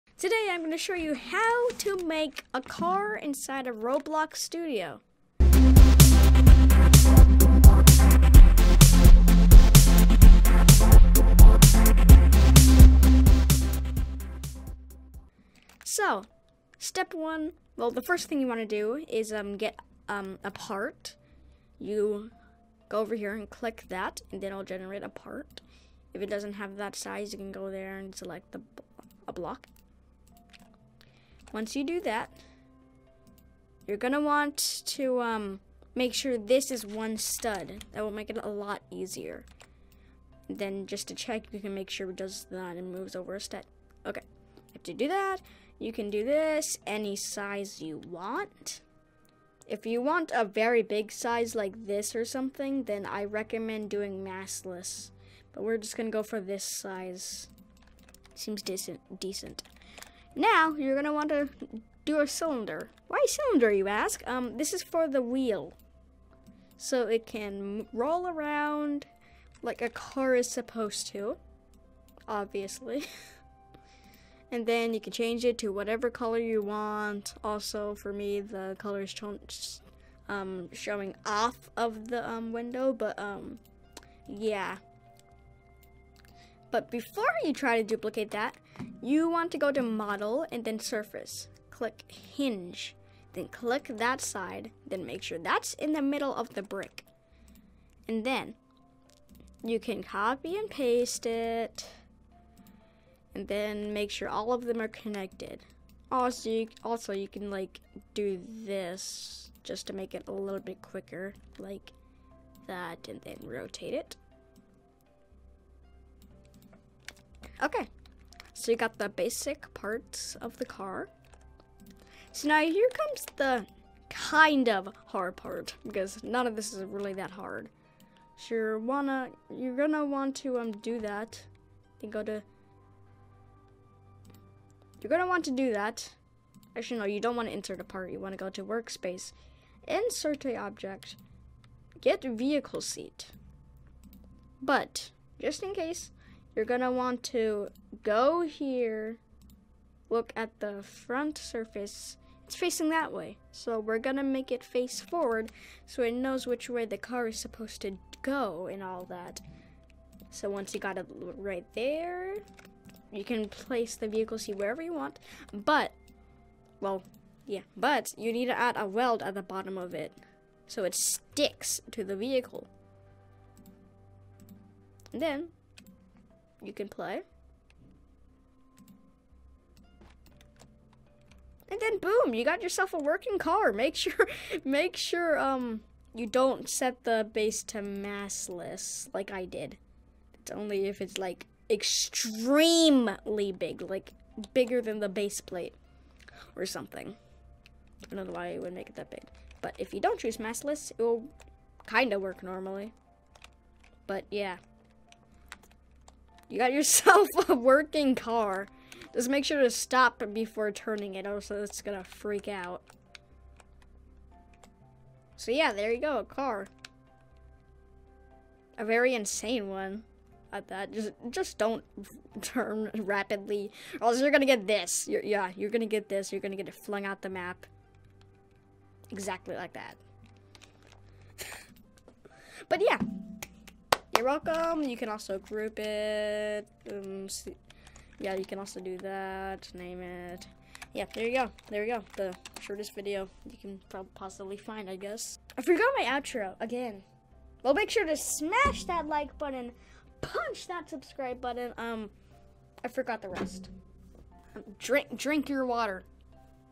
How to Make a Car in Roblox Studio
➡  I just checked on 1/1/2021 and it still works!
🔴*IMPORTANT* Roblox Changed The Hinge Feature. 🔴
 How to Hinge?
- Select your "Part"
- Go to "Properties"
- Then go down to "Surface"
- Now select the Surface you want to hinge, and change it from Smooth to Hinge.